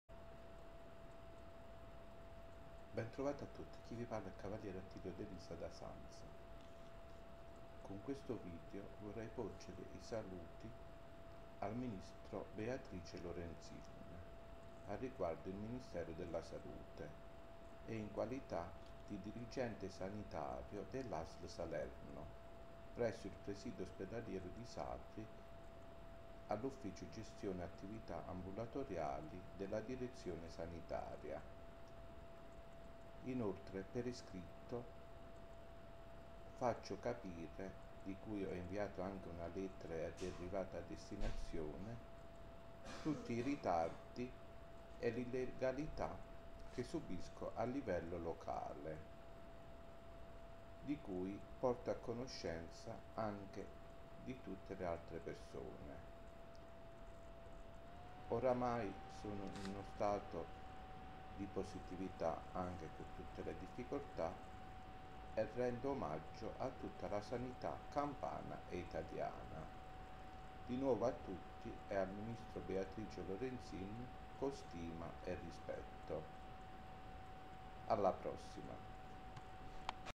Sanità Italiana: Beatrice Lorenzin - Nob. Cav. Attilio De Lisa - A.S.L. Salerno P.O. Sapri
Ministero e Ministro della Salute-Regione Campania. Lettera inviata e arrivata al Ministro Lorenzin. Oggetto: comunicazione al riguardo la posizione e situazione che il Nobile Cav. Attilio De Lisa di Sanza della Diocesi di Teggiano-Policastro (appartenente sia all’Ordine Equestre Pontificio di San Silvestro Papa della Santa Sede riconosciuto dallo Stato Italiano con a capo il Gran Maestro Papa Francesco Vescovo della Diocesi di Roma e Pontefice della Chiesa Cattolica) quale Referente del Governo delle Agende e delle Liste di Attesa dell’Ospedale Immacolata di Sapri presso la Direzione Sanitaria all’ufficio gestione delle Attività ambulatoriali fruttifero per l’Azienda che occupa e le negatività che è costretto a subire a livello locale con tutto in simbiosi e ben voluto a livello centrale ed oltre (di cui la stessa sede centrale ed oltre ne sono al corrente da tempo dopo comunicazione verbale e scritta).
Infatti da più di due anni ritardi anche con consenso del Direttore Sanitario Dott.ssa Maria Ruocco al riguardo la richiesta fatta varie volte all’Economato e all’Ufficio Tecnico (di cui il Direttore Amministrativo è l’Avv. Marilinda Martino che ha dato pure parere favorevole e fatte nel mese di: febbraio 2014,aprile 2014 e settembre 2015 con tanto di n. di protocollo generale e sanitario) una tabella identificativa dell’Ufficio di competenza (da posizionare al piano terra con l’indirizzo che è già visibile a livello di sito centrale dell’A.S.L. Salerno nel dovuto P.O. appartenente dove per merito sempre elogiato dalla Sede Centrale di Salerno e avendo lo stesso Cav. De Lisa anche i requisiti per svolgere anche mansioni superiori) come tutti gli altri e tutto per la visibilità e il diritto del Cliente ambulatoriale di sapere che al quarto piano esiste un ufficio dove rivolgersi al riguardo per ogni evenienza e fino adesso con esito negativo. Il Cav. De Lisa mai ha ricevuto una risposta scritta al merito ne da parte del Direttore Amministrativo che dall’ufficio Economato (neanche notizia dal suo Direttore Sanitario) e dopo aver interpellato il suo legale e portato a conoscenza dello stesso D.S.,è stato costretto ad acquistarla a proprie spese. Ma dopo circa un mese di esposizione al pubblico e all’insaputa sia del Cav. De Lisa che del D.S. dopo averlo interpellato e proprio nel giorno non lavorativo dell’ufficio di competenza (sabato-domenica),qualcuno si è permesso di toglierla e di posizionarne un’altra allo stesso posto e stessa misura con tanto spazio a disposizione (inviate foto prima e dopo allo stesso Ministro della salute oltre alla sede Centrale A.S.L. Salerno e Segreteria Presidente Vincenzo De Luca Regione Campania). Comunque dal 17 dicembre 2016 è stata consegnata allo stesso Direttore Sanitario un’altra tabella aggiornata in quanto il 16 dicembre 2016 ha partecipato al corso di formazione aziendale per le Liste di Attesa con il Ref. Aziendale Dott.ssa Maria Immacolata Borrelli oltre da quasi due anni già partecipato al nuovo AREAS Prenotazioni CUP Aziendale con il Resp. Dottor Carmine Mangino e dopo circa 5 mesi e quindi fino all’ 8 aprile 2016 lo stesso Direttore Sanitario si è rifiutato di farla posizionare di conseguenza riflettendo col pensare cose strane al riguardo in quanto prima ha acconsentito e tuttora si rifiuta andando nel verso dell’Ufficio Economato di Area Amministrativa che non l’ha acquistata così da violare anche il diritto del Malato. Ritardi anche nel dare l’autorizzazione (richiesta) di utilizzare il protocollo informatico aziendale che già lo utilizzano in tutta l’Azienda tranne Sapri oltre alle richieste fatte allo stesso D.S. per quanto riguarda ad altri uffici della stessa Direzione Sanitaria e non portate a termine come quella all’Ufficio Infermieristico al riguardo l’elenco degli addetti agli ambulatori ed altri uffici richiesto di trasferirgli tutto ciò che riguarda le attività ambulatoriali e mai fatto infatti oggi i resoconti mensili ambulatoriali ancora pervengono agli stessi. Comunque la cosa positiva che con tutte le difficoltà illegali incontrate e continua ad incontrare,ha creato una rete ambulatoriale locale con la segreteria della Direzione Sanitaria e i vari servizi e reparti ambulatoriali (ognuno con un proprio Referente) al rispetto delle Linee Guida e delle Disposizioni Centrali.